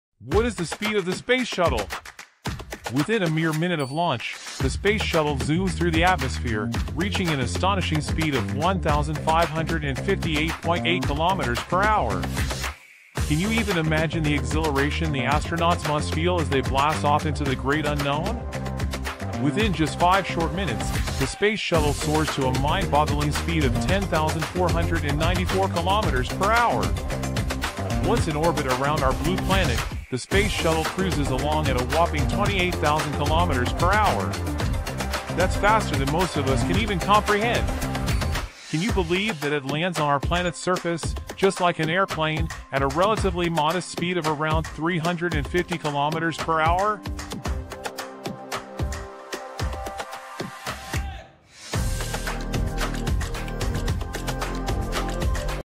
What is the Speed of a Space Shuttle?
What is the speed of the space shuttle?

Imagine this: to reach the minimum altitude required to orbit our beautiful Earth, the space shuttle goes from a standstill to a jaw-dropping speed of 8,000 meters per second. That's nearly 18,000 miles per hour! And guess what? It accomplishes this incredible feat in just eight and a half minutes. Talk about an adrenaline rush!

But hold on tight, because the adventure doesn't end there. Within a mere minute of launch, the space shuttle zooms through the atmosphere, reaching an astonishing speed of 1,558.8 kilometers per hour. Can you even imagine the exhilaration the astronauts must feel as they blast off into the great unknown?

And it doesn't stop there! Within a blink of an eye, just five short minutes, the space shuttle soars to a mind-boggling speed of 10,494 kilometers per hour. That's like crossing vast distances in the blink of an eye!

Now, here's a fun fact: once in orbit around our blue planet, the space shuttle cruises along at a whopping 28,000 kilometers per hour. That's faster than most of us can even comprehend! But when it's time for the shuttle to return home, it gracefully enters the Earth's atmosphere, gradually slowing down. Can you believe that it lands on our planet's surface, just like an airplane, at a relatively modest speed of around 350 kilometers per hour? It's a true testament to the precision and engineering marvels of space exploration.

So, my incredible viewers, let's take a moment to appreciate the awe-inspiring speed of the space shuttle, from its breathtaking launch to its graceful return. The wonders of space travel never fail to leave us in awe. Until our next cosmic adventure, keep exploring and keep reaching for the stars!

Video Credits - NASA.